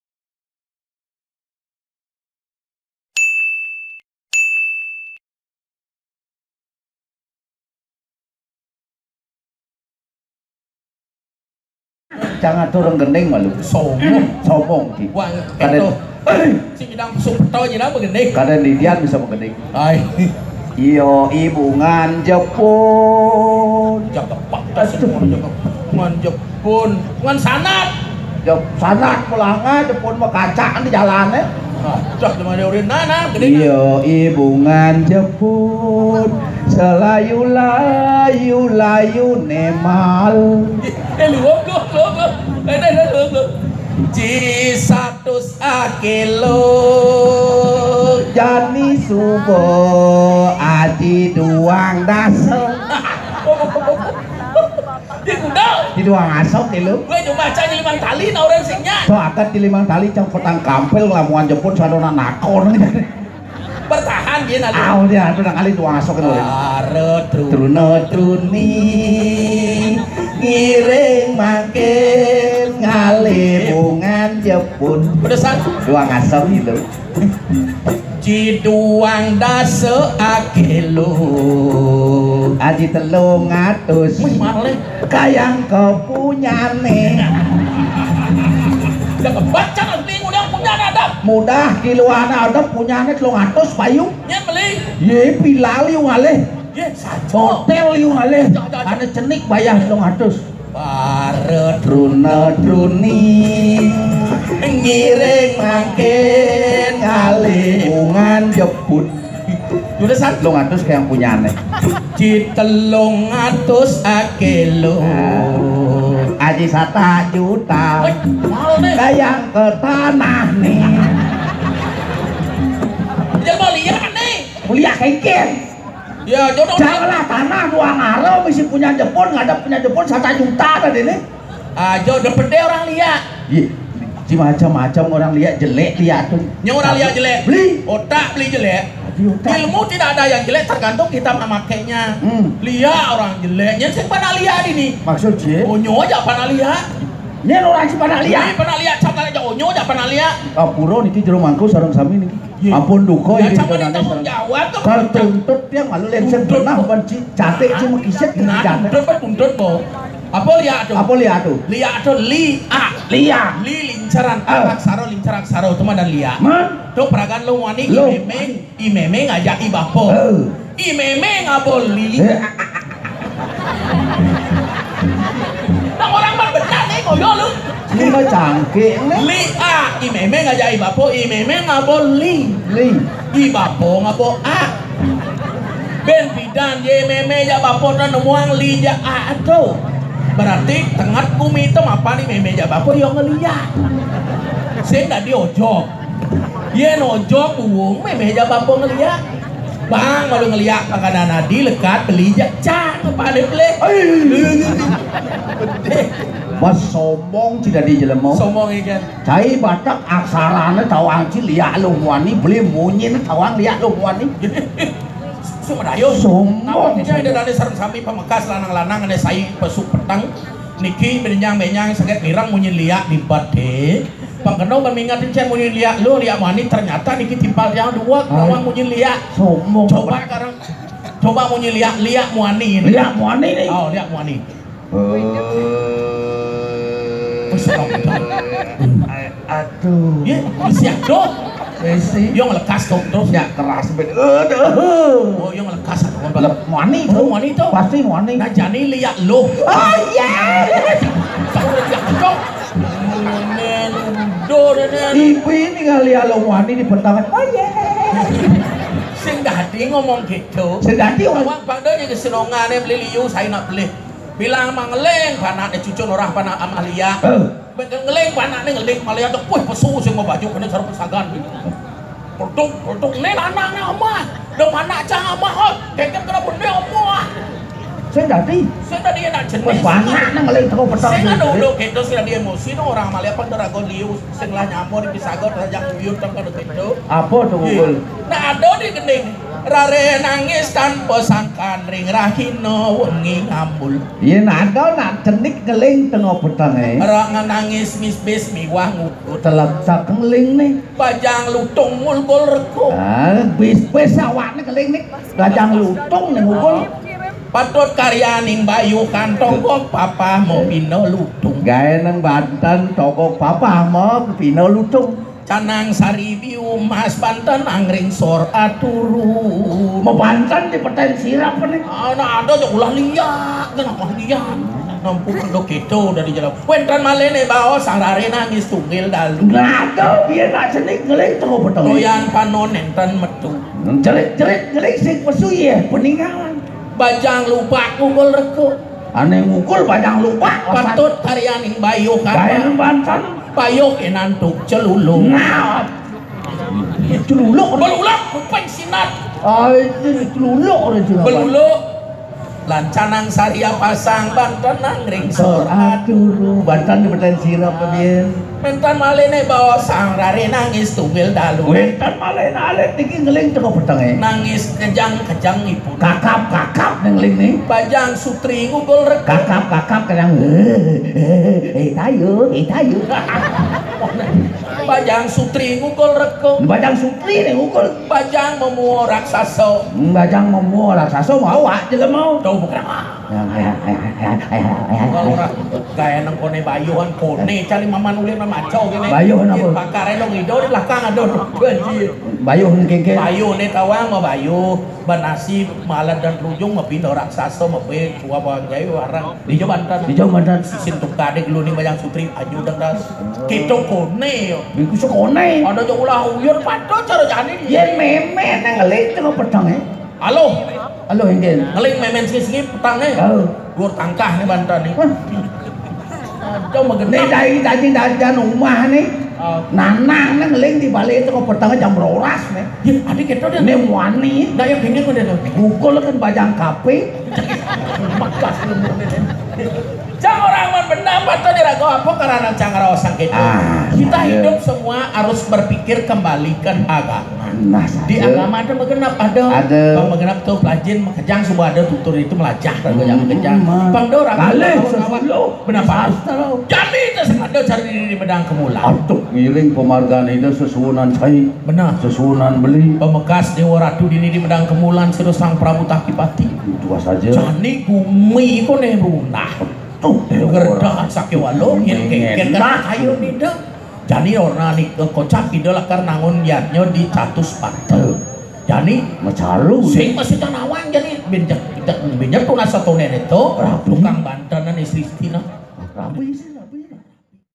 ARJA Akah Canging | imeme jak Bapa NGELIAK di KAMAR
Lawakan Bali ini berpariasi dan sangat bagus lucu..